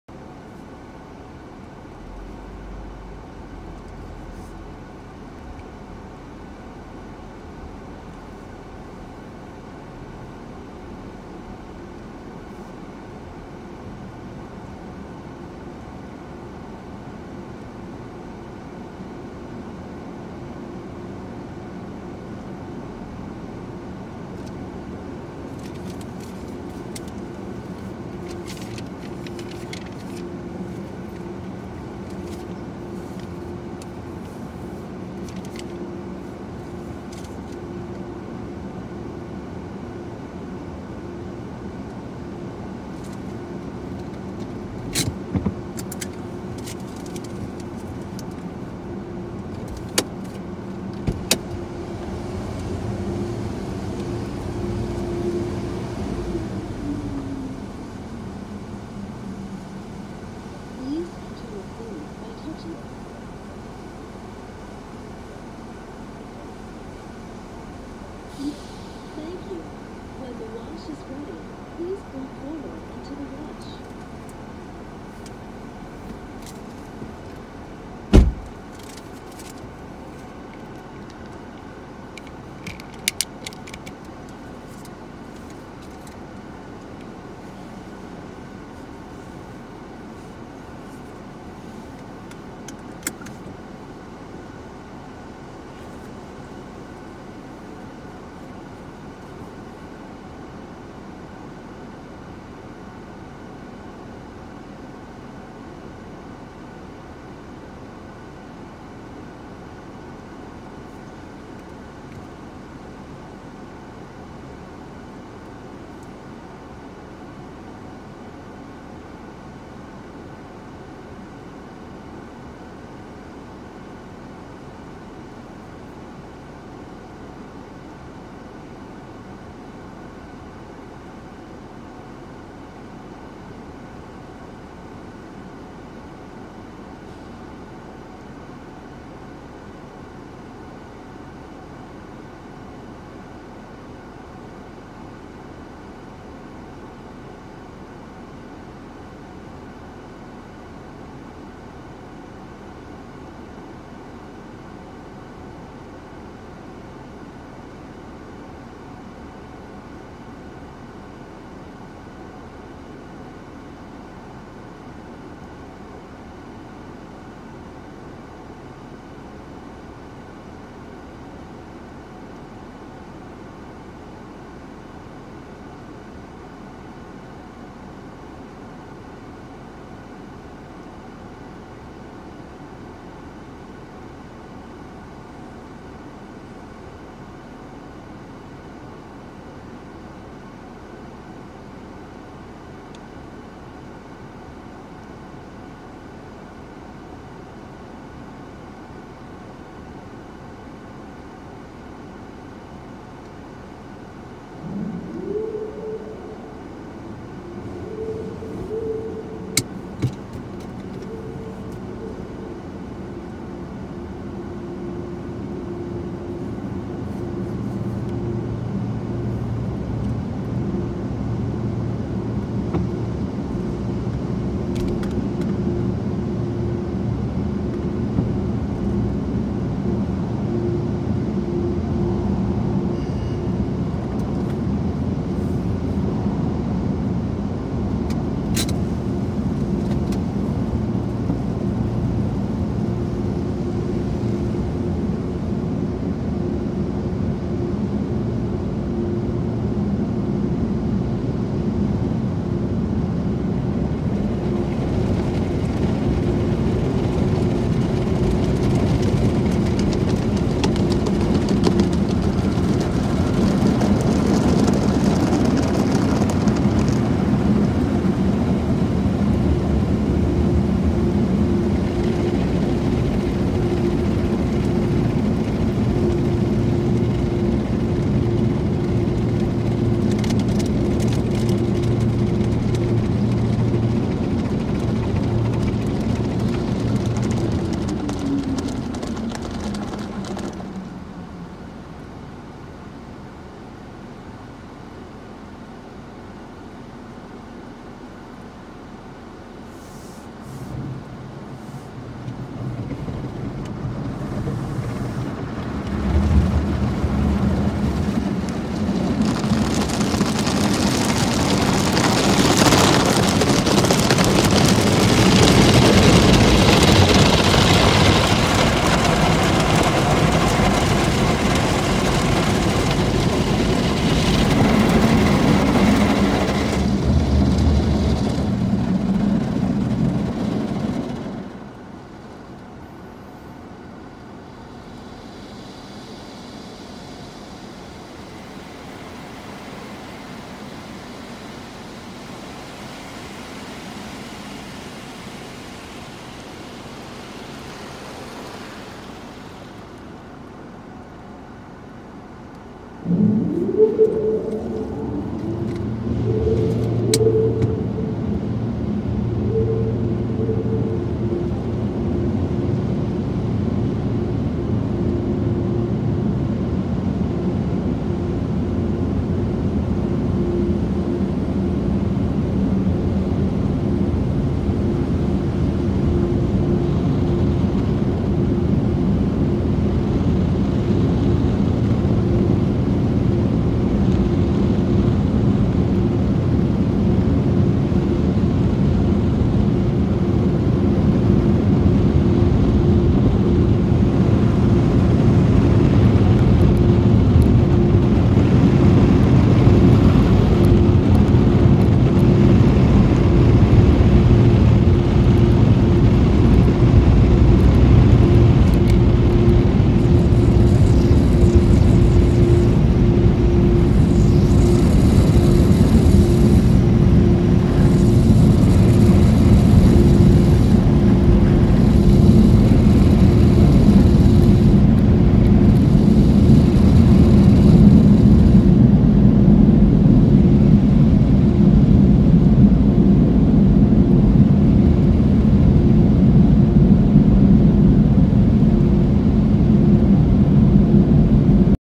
Out and About: Automated Car Wash (RAW VIDEO)
Day in the life of Assetto. He takes his car to an automated car wash cuz he wants a clean car

1.  Foul language
2.  Spam
3.  Bullying to other individuals
4.  Racism and sexism. (No sex jokes)